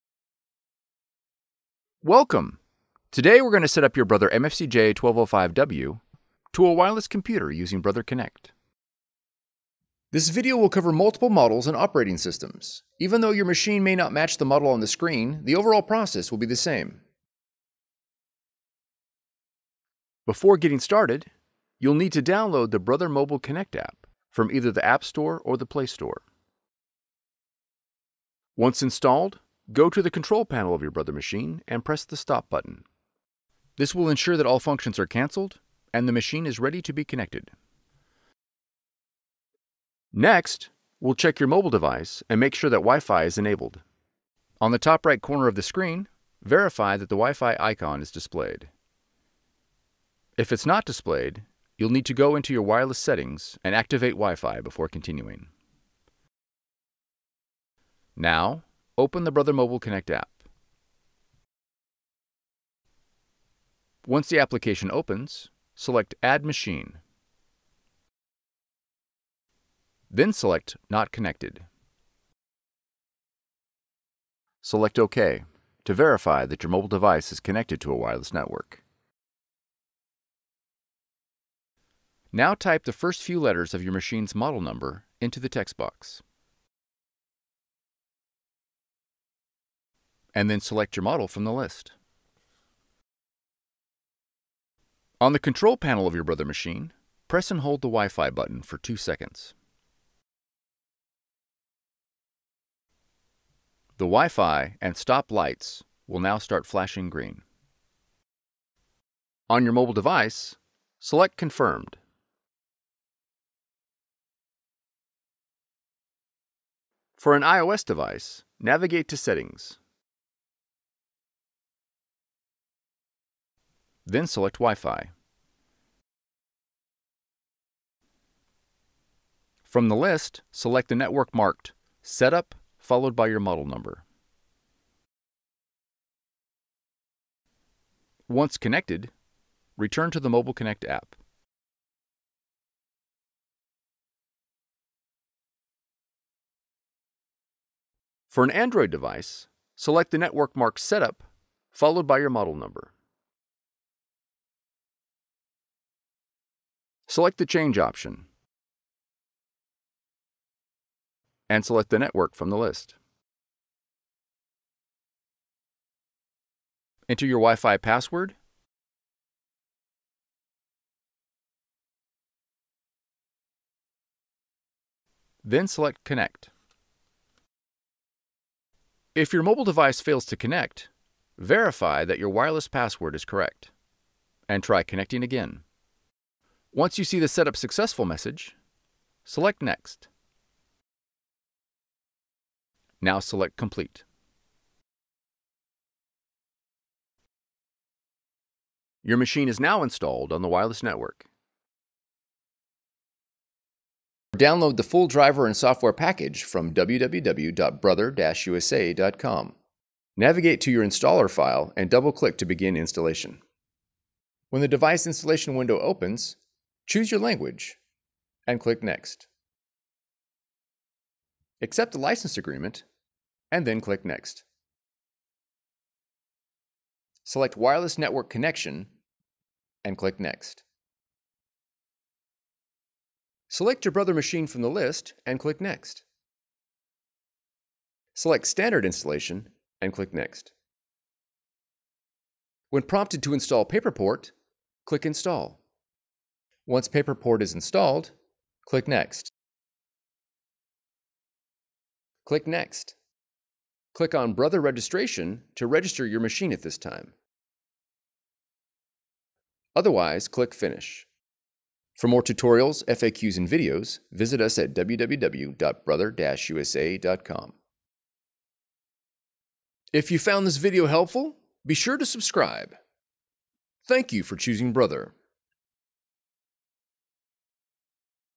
MFCJ1205W wireless setup - Windows
Learn how to set up your Brother MFC-J1205W inkjet printer on a wireless network in this video tutorial. This method will focus on using Brother Mobile Connect to establish the wireless connection.

Models covered:
Brother MFCJ1205W
Brother MFCJ1205W XL

Chapters:
Intro 00:00
Models covered 00:10
Download Mobile Connect 00:20
Verify device Wi-Fi  00:40
Open app and add machine 01:00
Enter Model number 01:20
iOS steps 02:00
Android steps 02:30
Download the software 03:20
Running the Installer software 03:31
PaperPort Install 04:03
User’s Guide and Registration 04:15

Transcript:
Welcome, today we’re going to earn how to set up your Brother MFCJ1205W to a wireless computer using Brother Connect

Before getting started you will need to download the Brother Mobile Connect app from the app store or the play store. Once installed, go to the control panel of your brother machine and press the stop button. This will ensure that any functions are canceled and the machine is ready to be connected.

Next, we’ll check your mobile device and make sure that WiFi is enabled. On the top right corner of the screen verify that the wifi icon is displayed. If it is not displayed you will need to go into your wireless setting and turn wifi on before continuing.

Now open Brother mobile connect app. Once the application opens, select add Machine.

Then select Not Connected. Select OK to verify that your mobile device is connected to wireless network.

Now type the first few letter of your machine’s model number in the text box and then select your model from the list. (possible split screen – use standard machine image with replaceable number)

On the control panel of your Brother machine press and hold the Wi-Fi button for two seconds. The
WiFi and the STOP lights will now start flashing green.

On your mobile device, select Confirmed

For an iOS device, navigate to settings. Then select WiFi. From the list, select the network marked SETUP followed by your model number.  Once connected, return to the Mobile Connect app.

For an Android device, select the network marked SETUP followed by your model number. Select the Change option. And sect your network for the list

Enter your WiFi password then select Connect

If your mobile device fails to connect, verify your wireless is password is correct and try connecting again.
Once you see the Setup Successful message, select Next.

Now select Complete

Your Brother machine is now installed on the wireless network.

Download the full driver and software package Navigate to your installer file and double-click to begin installation.

Select "Wireless network connection (Wi-Fi)" and click "Next"

To install PaperPort click "Install."

Click "Next"

Click on Brother registration to register your machine at this time otherwise click "Finish."

Customers also searched:
Brother MFC J1205W wireless Windows
Brother MFC J1205W wireless setup Windows